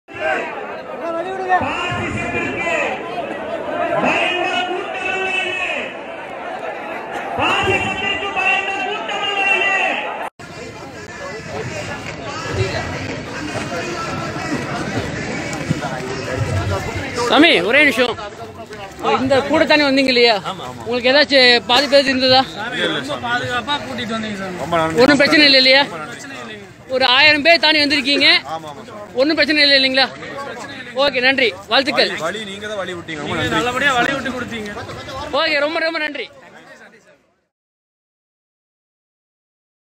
போராட்டத்துக்கு நடுவே ஐயப்ப பக்தர்களுக்கு வழிவிட்ட Muslims | CAA Protests
கோவையில் குடியுரிமை திருத்த சட்டத்துக்கு எதிராக போராட்டம் நடத்திய முஸ்லிம்கள், போராட்டம் நடத்திய கூட்டத்தினரைக் கடந்து சென்ற ஐயப்ப பக்தர்களுக்கு இடையூறு இல்லாமல் வழிவிட்டனர்.

இந்த காணொளி சமூக வலைத்தளங்களில் பரவலாகப் பகிரப்பட்டு வருகிறது.